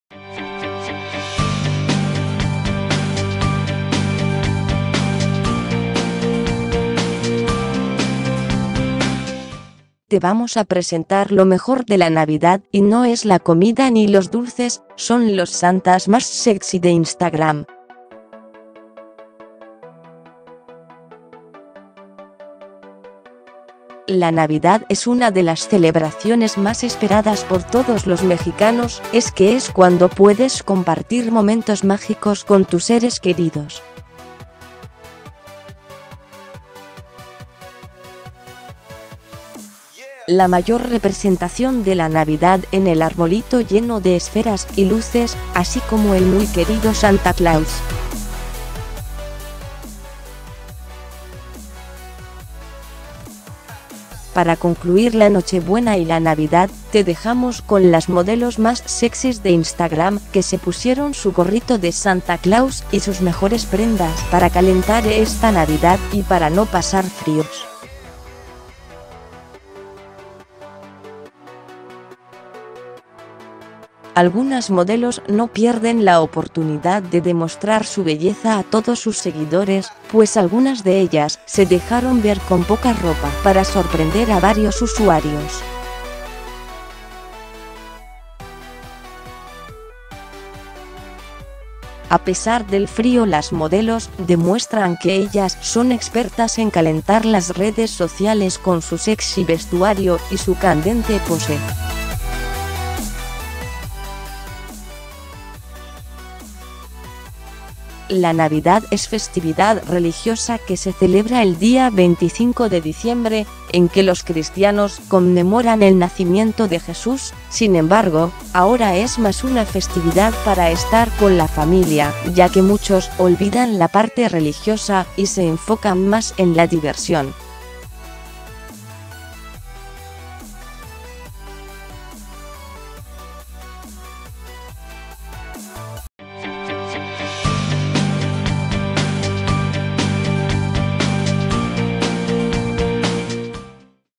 Santas sexy prenden Navidad en Instagram
La Navidad es una de las celebraciones más esperadas por todos los mexicanos es que es cuando puedes compartir momentos mágicos con tus seres queridos. La mayor representación de la Navidad en el arbolito lleno de esferas y luces, así como el muy querido Santa Claus.

Fuente: goo.gl/jVzFfA